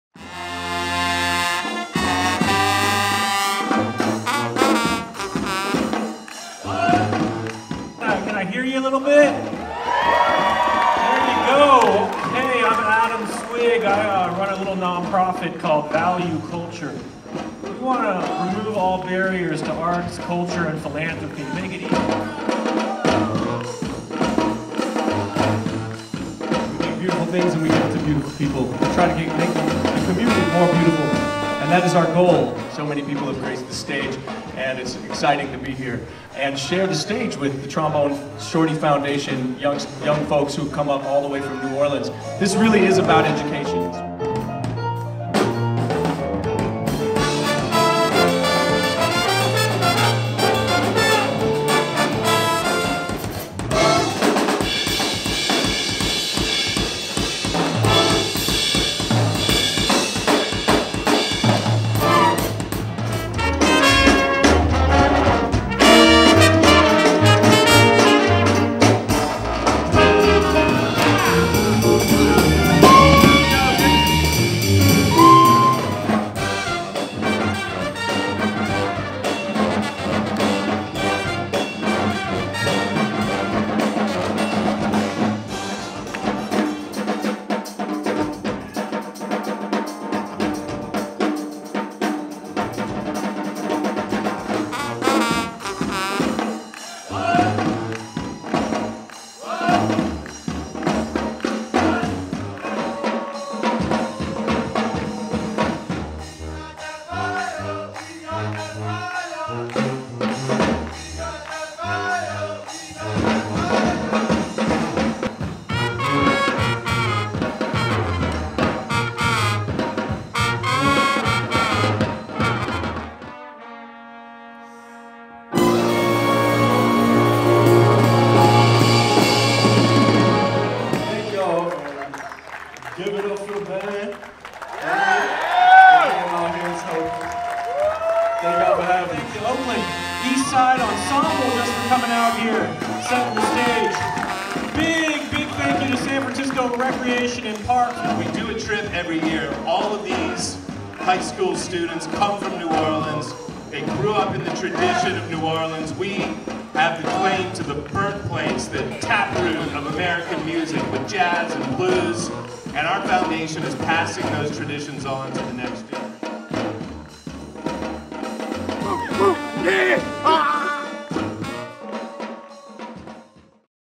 Trombone Shorty Academy & Oakland EastSide All-Star Ensemble, San Francisco 2022 Live
Trombone Shorty Academy from New Orleans play a free show at the Bandshell in Golden Gate Park with the Oakland EastSide All-Star Ensemble

Trombone Shorty's @tromboneshorty Trombone Shorty Foundation
@shortyfoundation was in the San Francisco giving their high school kids The Trombone Shorty Academy the experience of a lifetime, to rock the stage with fellow high school group Oakland EastSide All-Star Ensemble @oease.official from across the bay to play at this historic venue!
Support the next generation of artists with us!

Presented by Illuminate the Arts @illuminatethearts San Francisco Recreation and Parks @sfrecpark San Francisco Parks Alliance @sfparksalliance in association with Value Culture @ivalueculture & SF Jazz @sfjazz !!!